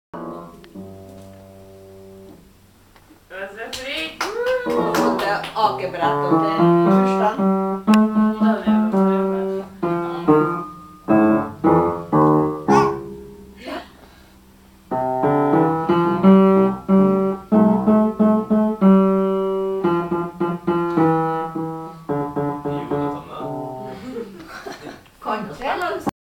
Jonathan_Sandra.MOV
Jonathan spiller piano sammen med tante Sandra